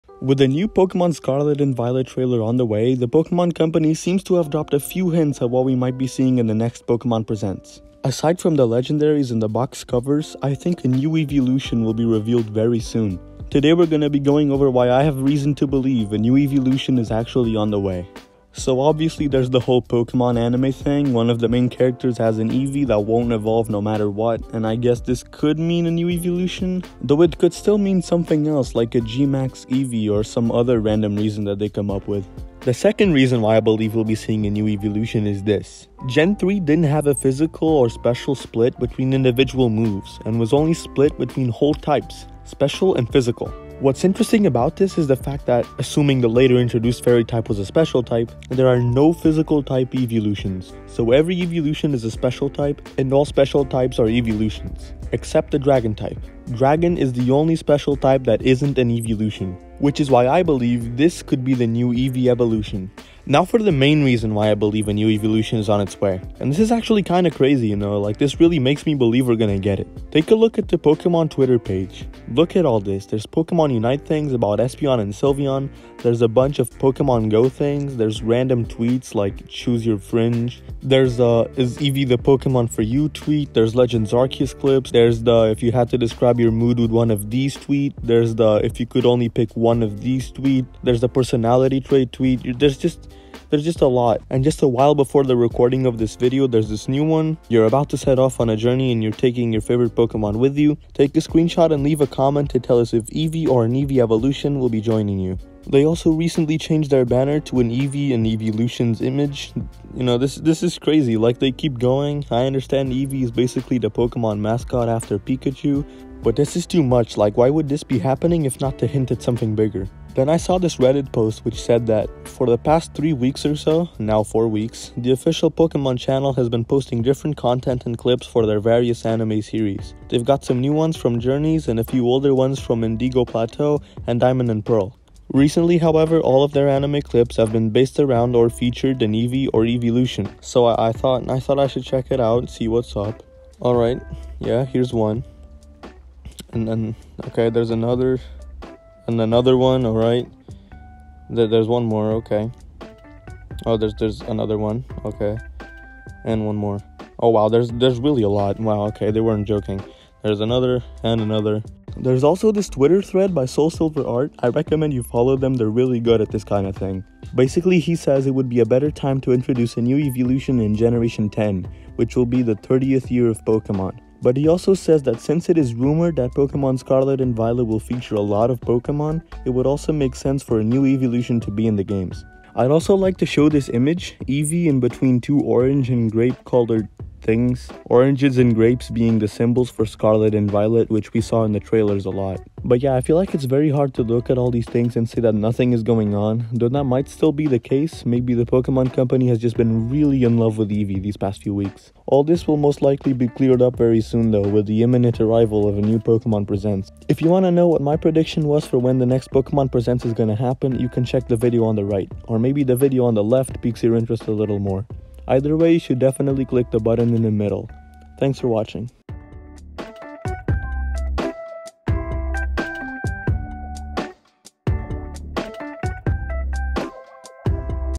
NEW EEVEELUTION! | Pokemon Leaks and Rumors Discussion
With every new generation comes the possibility of a new Eeveelution.
In this video we're going to go over the hints and teasers made by the Pokemon Company in favor of a new Eeveelution for Pokemon Scarlet and Violet!